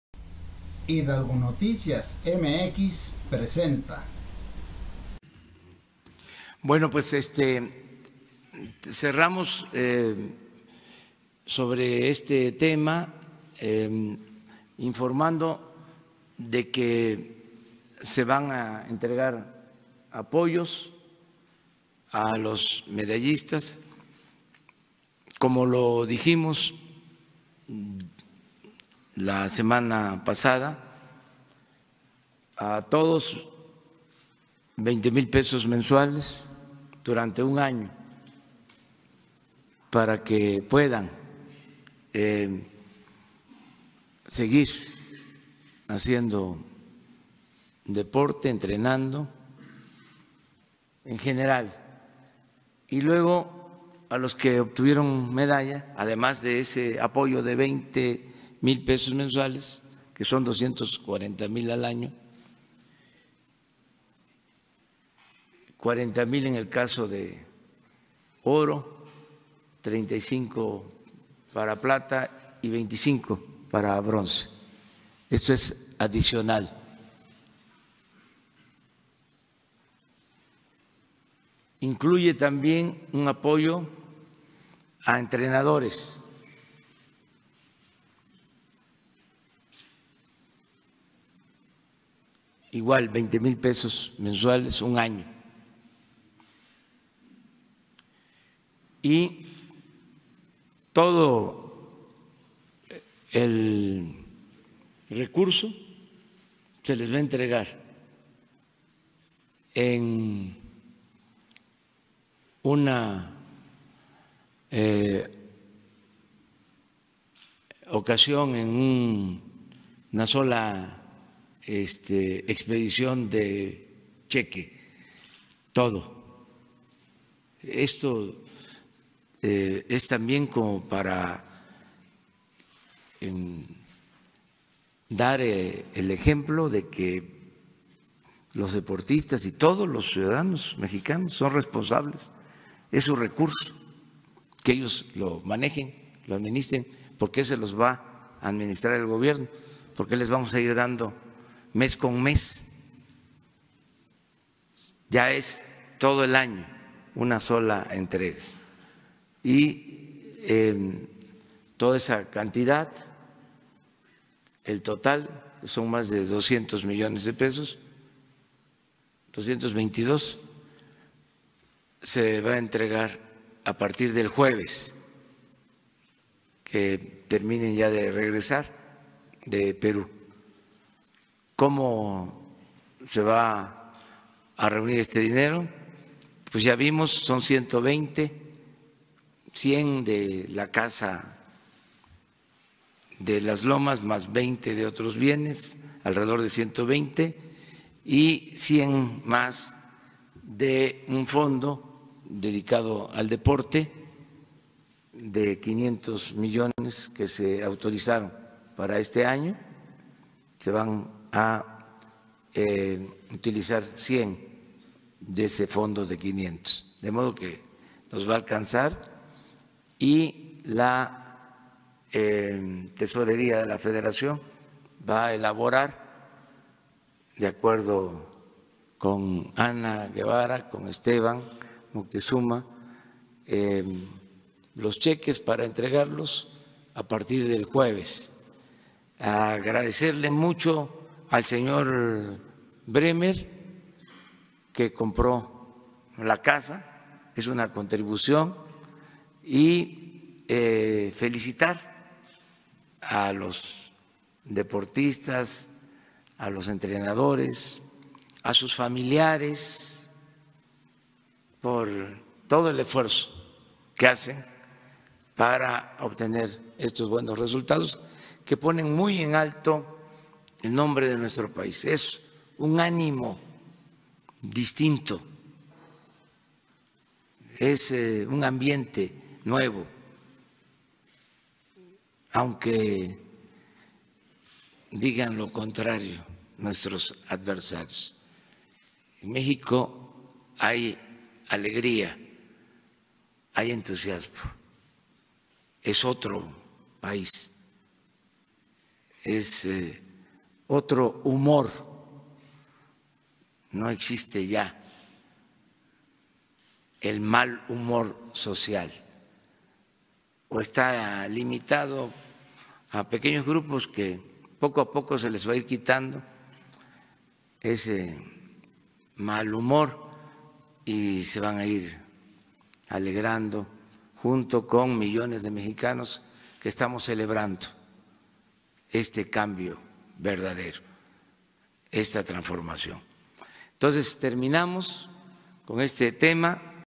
AMLO detalló los apoyos a deportistas que acudieron a los Panamericanos y a los que ganaron medallas
El Presidente explicó como se les darán los apoyos económicos a los atletas que acudieron a los Juegos Panamericanos y el apoyo adicional a los que ganaron medallas.